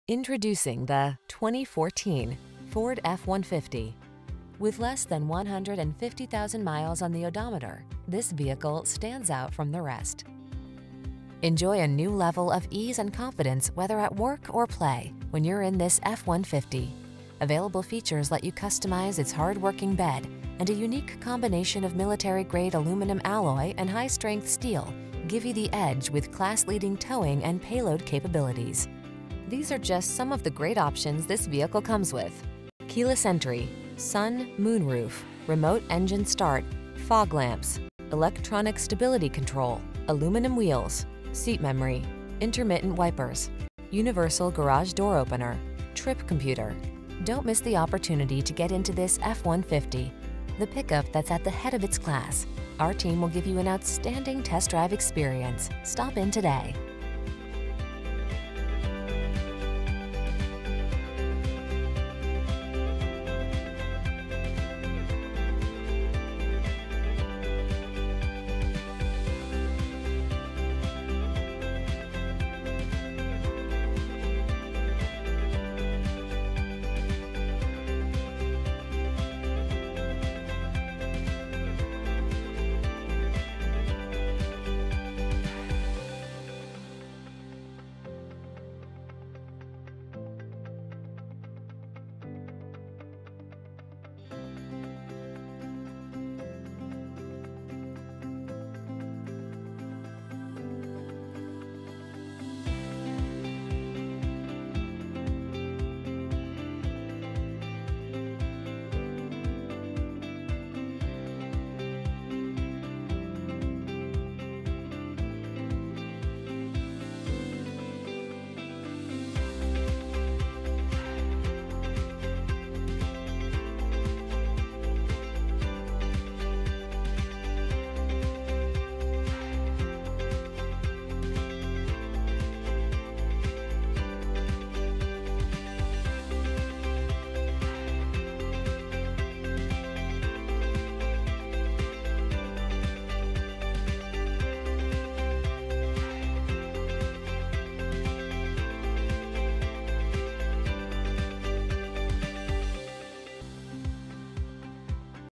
2014 Ford F-150 Woodstock, Crystal Lake, McHenry, Huntley, Lake in the Hills, IL 12426B
Green Gem Metallic Used 2014 Ford F-150 available in Woodstock, Illinois at Bull Valley Ford. Servicing the Crystal Lake, McHenry, HUntley, Lake in the Hills, IL area.

1460 South Eastwood Dr.

2014 Ford F-150 Lariat, 4WD, 6-Speed Automatic Electronic, 3.5L V6, Equipment Group 502A Luxury, Heated front seats, Navigation, Power Moonroof, PowerFold Heated Side Mirrors, Rear-View Camera, Remote Start System, Reverse Sensing System.brbrRecent Arrival!